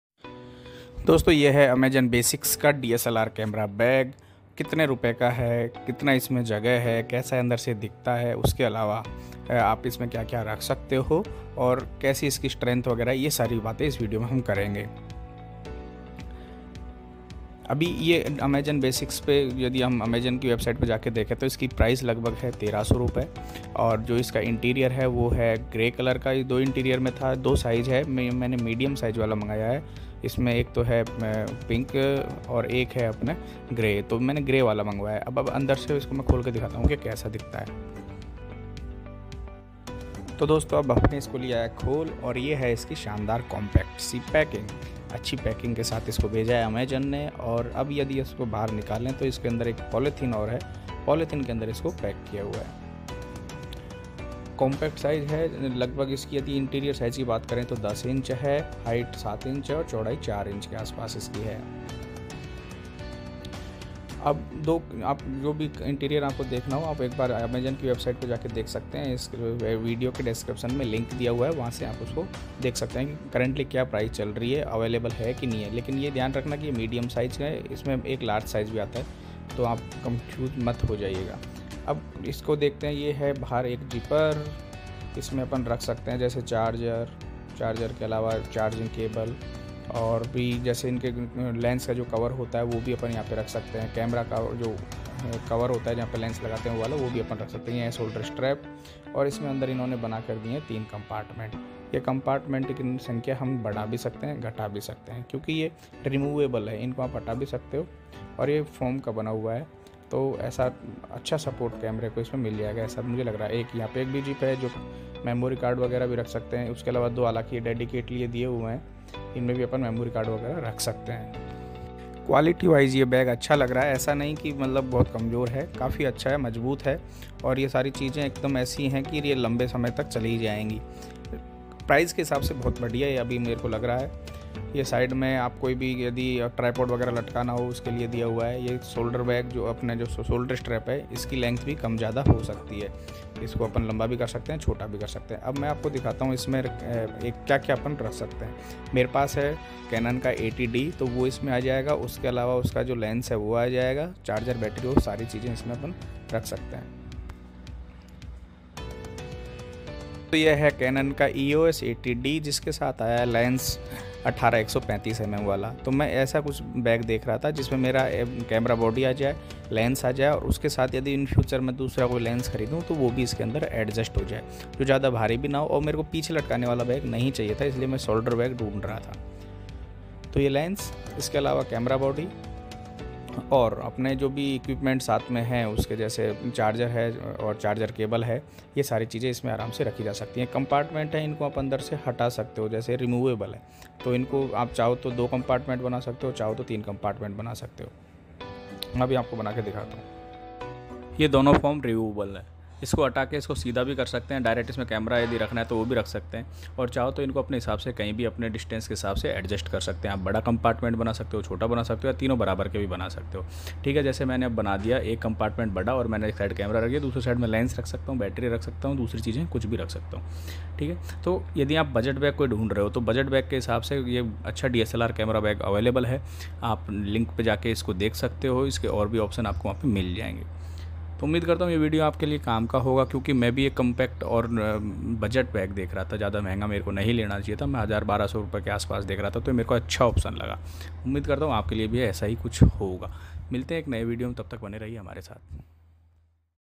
Amazon basics DSLR camera bag|| best DSLR camera bag in budget || canon Eos 80d Camera bag
Amazon basics camera bag for DSLR || Amazon basics medium DSLR camera bag || Budget Camera bag

price approx ₹ 1300
 we can say it is best budget bag for dslr camera.
For beginners amazon basics DSLR medium camera bag is great choice.
it is perfect choice Canon Eos 80d DSLR camera bag.